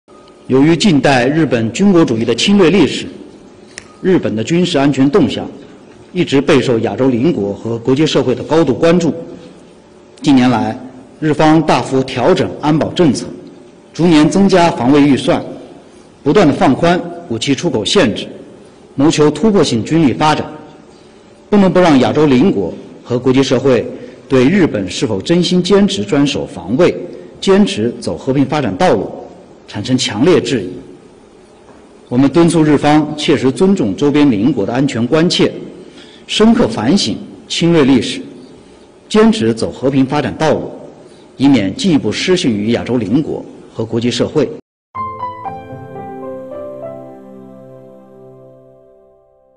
日本2024年度预算防卫费创历史新高 中方回应
中国外交部发言人林剑3月29日主持例行记者会。

有记者提问：据报道，28日，日本正式通过2024年度预算，其中防卫费达7.95万亿日元(约合4100亿元人民币)，同比增长16.9%，创下历史新高。请问中方对此有何评论？

林剑表示，由于近代日本军国主义的侵略历史，日本的军事安全动向一直备受亚洲邻国和国际社会高度关注。近年来，日方大幅调整安保政策，逐年增加防卫预算，不断放宽武器出口限制，谋求突破性军力发展，不能不让亚洲邻国和国际社会对日本是否真心坚持专守防卫、坚持走和平发展道路产生强烈质疑。我们敦促日方切实尊重周边邻国的安全关切，深刻反省侵略历史，坚持走和平发展道路，以免进一步失信于亚洲邻国和国际社会。